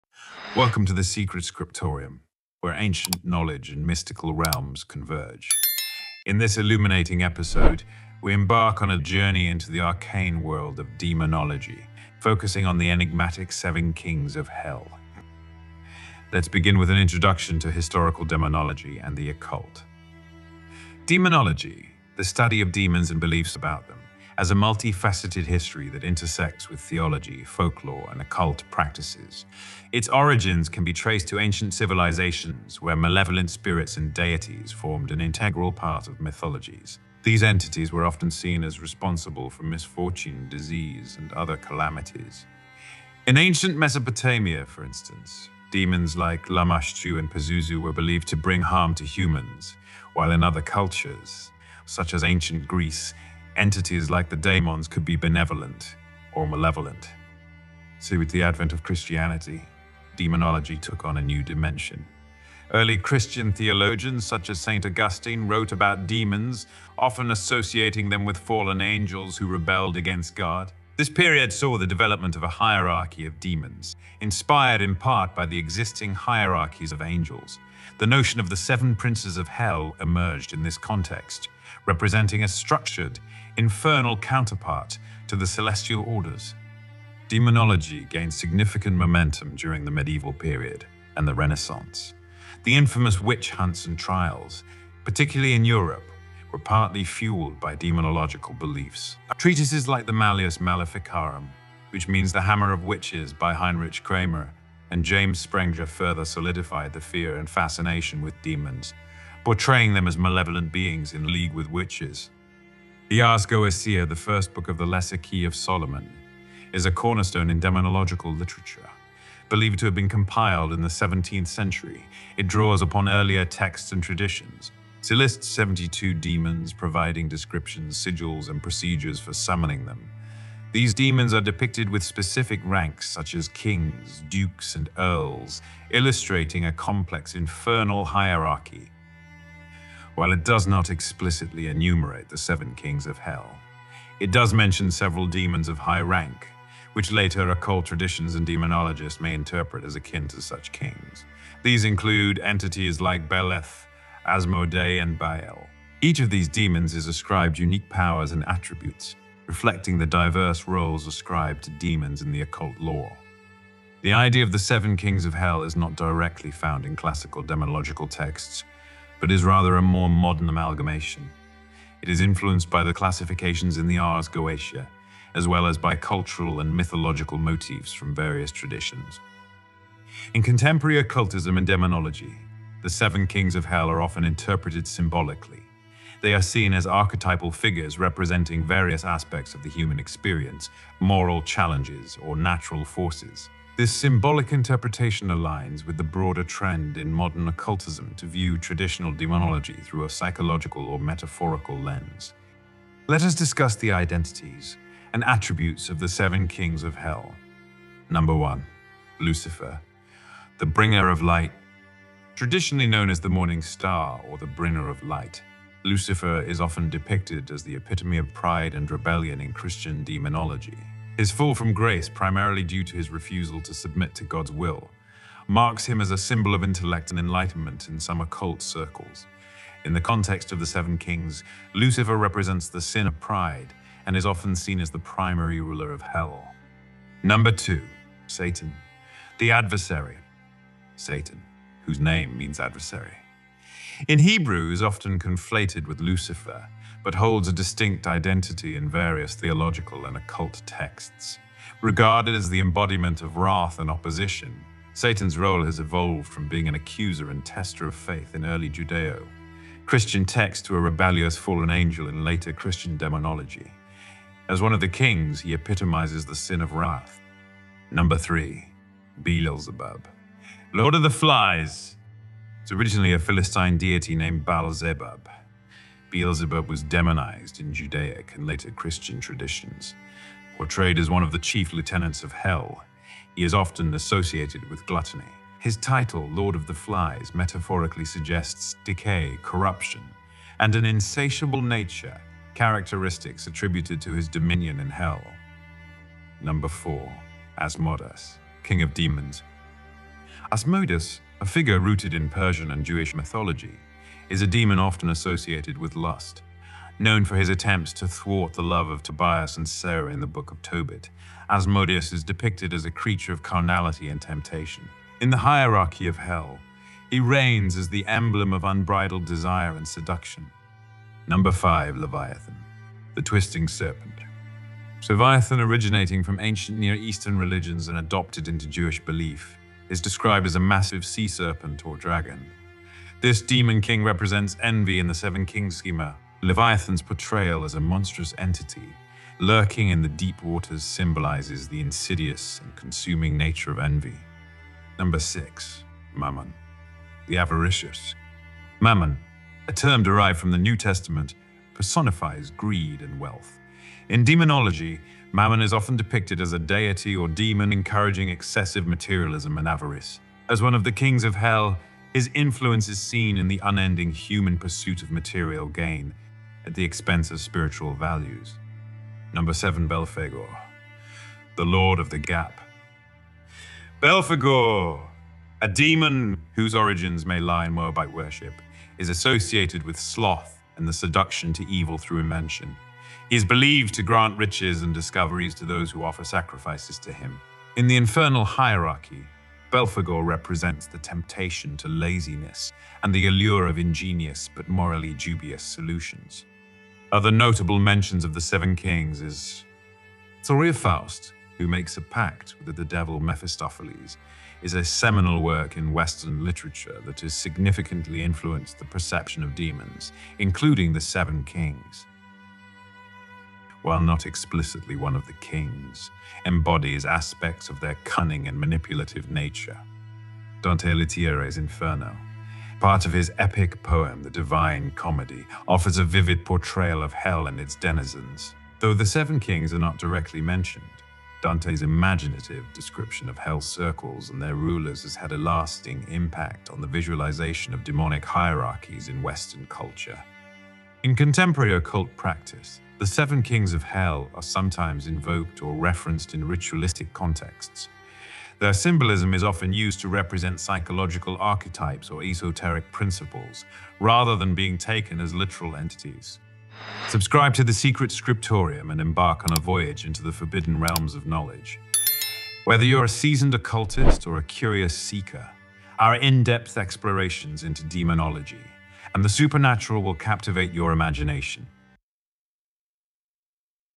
Demonology & The Seven Kings Of Hell
Audio & Video for personal use only. Copying, re-uploading, sampling etc. not permitted.

Welcome to The Secret Scriptorium, a sanctuary of ancient wisdom and esoteric knowledge. In this enlightening episode, we embark on a journey into the enigmatic realm of demonology, focusing on the elusive Seven Kings of Hell.

Join us as we delve deep into the profound field of demonology, a subject that has both fascinated and terrified humanity throughout the ages. Discover the historical origins of demonology, tracing its roots from ancient religious texts to the intricate system of classifying demons that exists today.

In this exploration, you'll meet the Seven Kings of Hell—Lucifer, Mammon, Asmodeus, Leviathan, Beelzebub, Satan, and Belphegor. Each of these infernal monarchs possesses unique dominions, powers, and symbolic significance across various cultural and occult traditions.

Uncover the pervasive presence of demons in global cultures, from the Middle East to Europe and beyond. Learn how these malevolent entities have influenced folklore, religious beliefs, and artistic expression throughout the annals of history.

Trace the origins of the Seven Kings, from their biblical beginnings as fallen angels to their evolution into rulers of the infernal realms. Gain insight into how these entities have been portrayed and interpreted in religious and occult literature over time.

Delve into the practical aspects of demonology, including exorcism rituals and the invocation of demons for various purposes. Examine the moral and ethical dilemmas that surround the study and practice of working with these dark forces.

Whether you're a seasoned practitioner of the arcane arts or a curious seeker of forbidden knowledge, our comprehensive investigations into demonology and the supernatural will leave you spellbound.

Unlock the gates to the abyss and unravel the enigma of the Seven Kings of Hell. Join us in decoding the mystifying world of demonology—a realm where shadows and illumination coexist in eternal fascination.